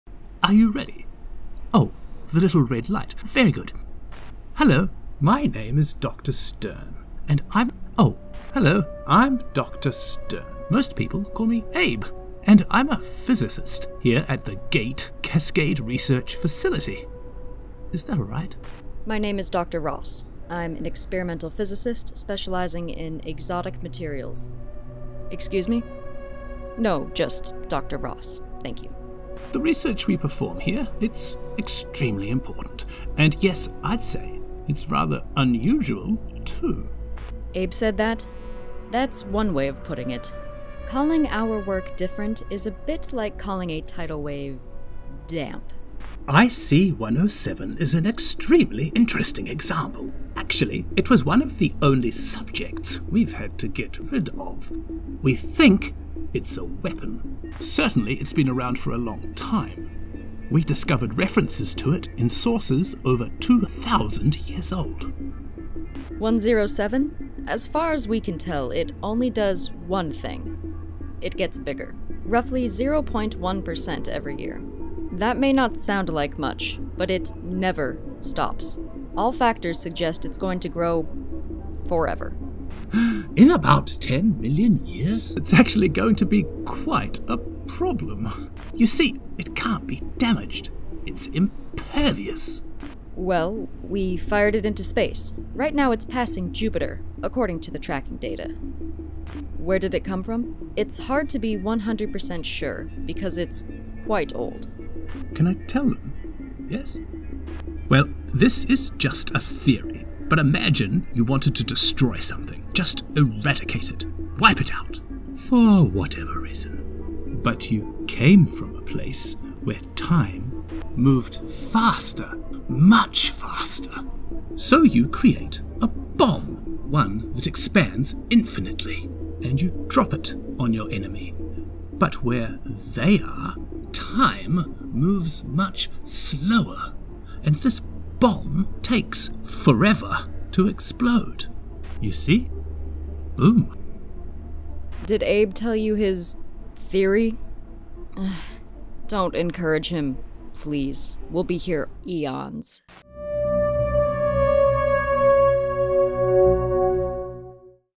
The Time Bomb | Archive Interviews
Watch our first "unusual artifact" documentary, from the Archives. For GATE employees only.

Developed by Deep Field Games.